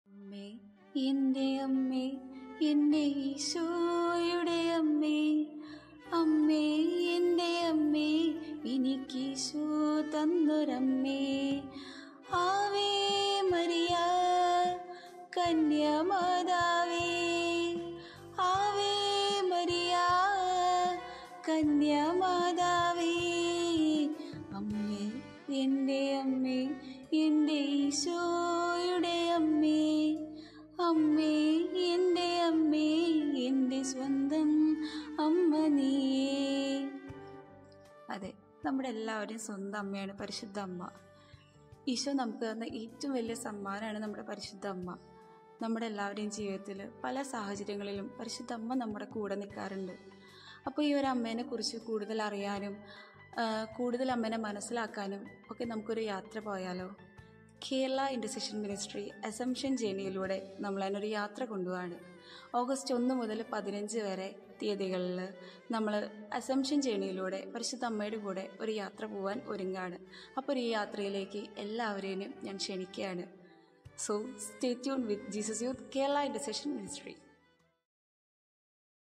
Assumption Journey
Hi  🙋🏻‍♀️
നമ്മുടെ യാത്ര ദേ ഇങ്ങ് അടുത്തെത്തിട്ടാ 🚆അപ്പൊ എങ്ങനെയാ അമ്മയോടൊപ്പം... അമ്മയോടു കൂടെ ആയിരിക്കാൻ... പ്രാർത്ഥിച്ചു ഒരുങ്ങാൻ കൂടെ കൂടല്ലേ .👭👫👭